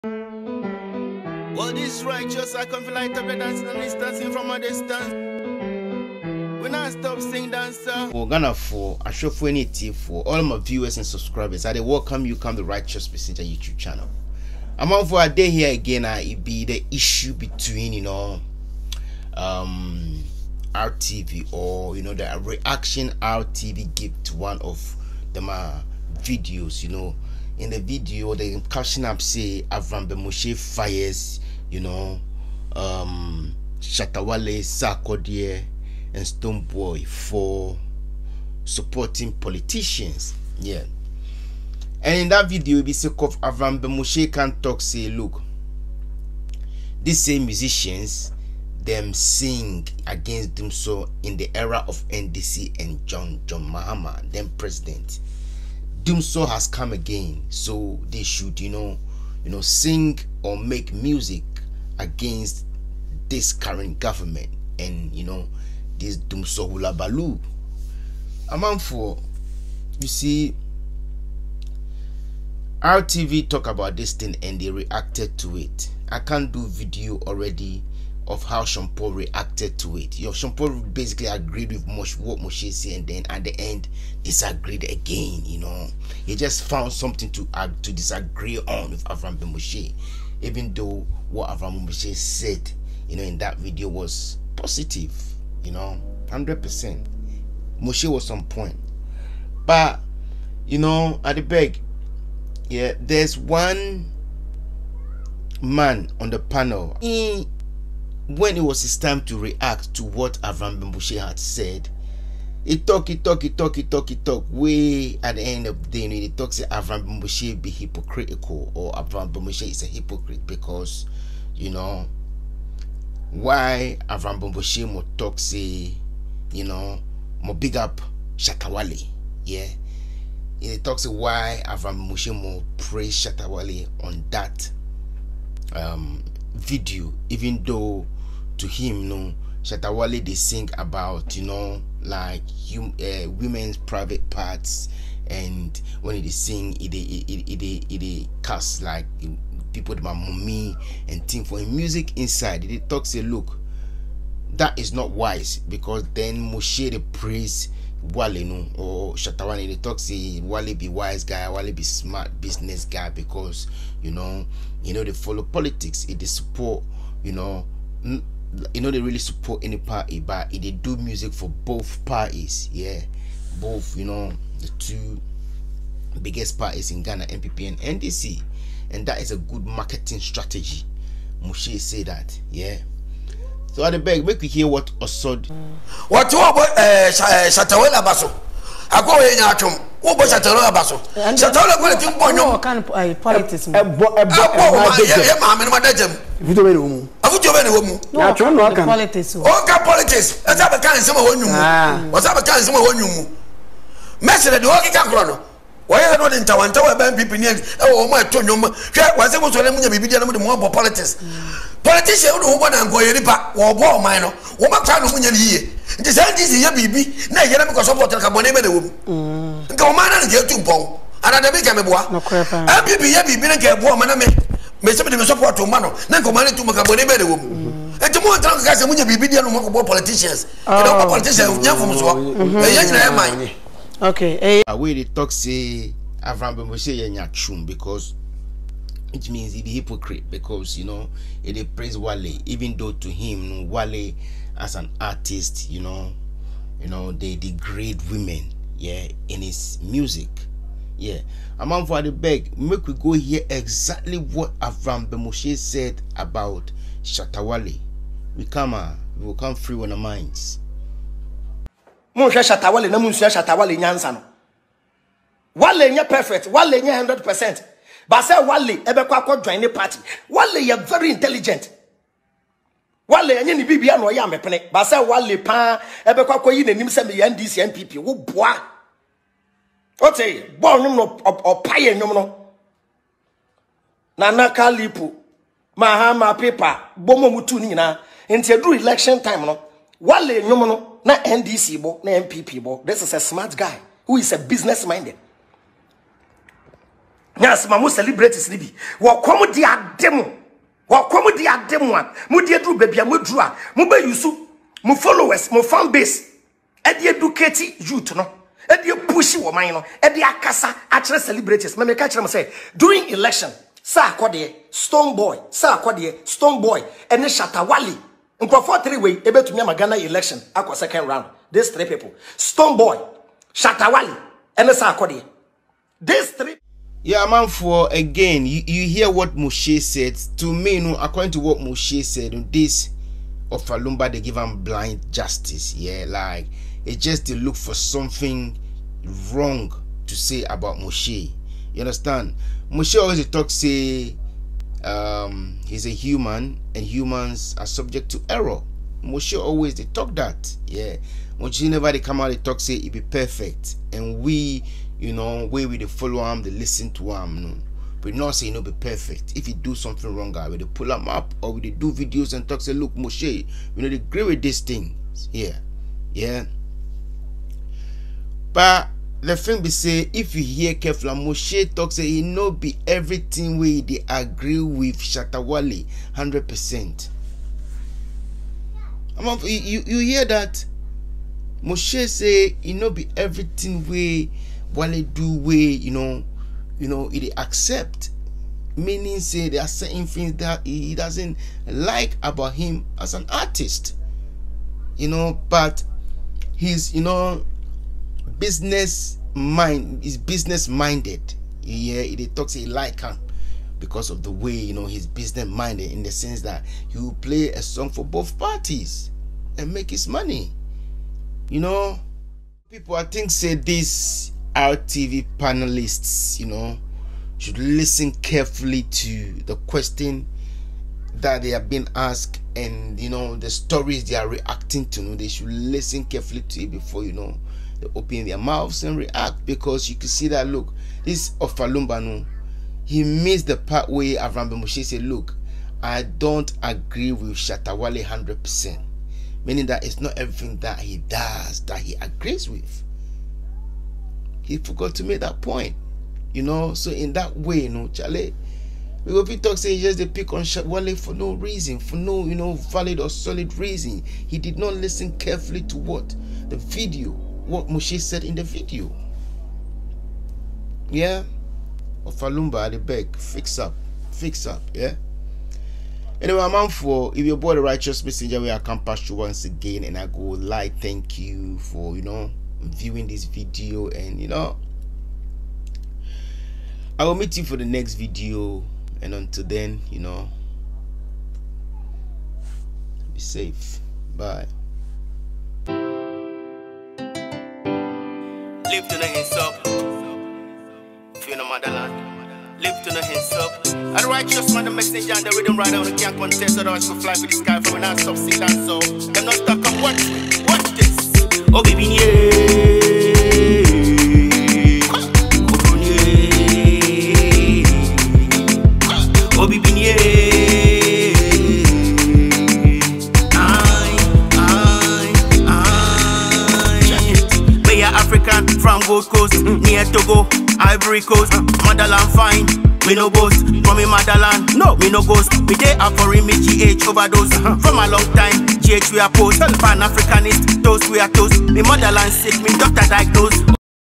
Osoode Of RTV Calls Avraham Ben Moshe A Hypocrite Bcos Of Shatta Wale | My Reaction
The Rychuhss Messenger reacts to Osoode of RTV calling Avraham Ben Moshe a hypocrite